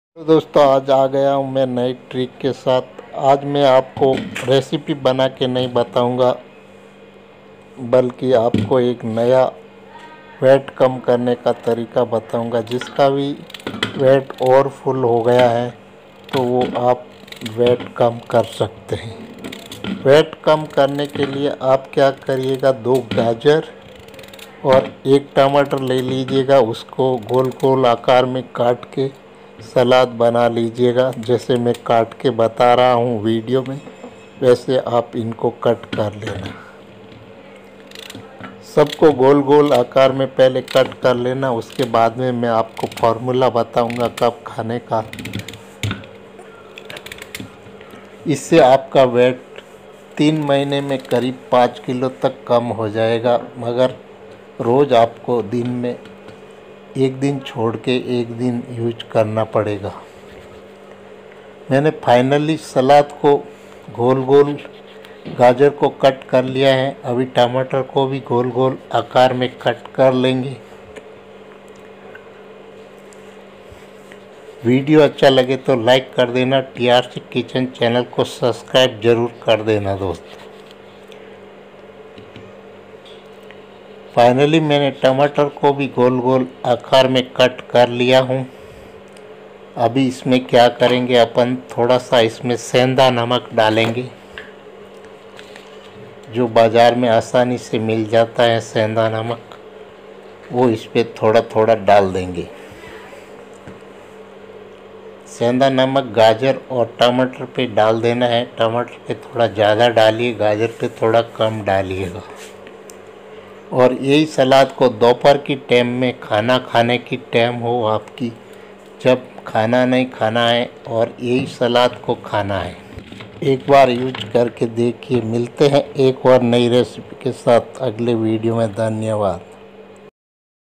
गाजर टमाटर की ऐसी रेसिपी कि आपका एक महीने में 5 किलो वेट कम कर सकते हैं
वेट कम करने के लिए गाजर टमाटर की रेसिपी

mahine mein 5 kg vate kam Karen
vate kam karne ka formula
vate kaise kam Karen

how to make Indian TRC kitchen Hindi recipe channel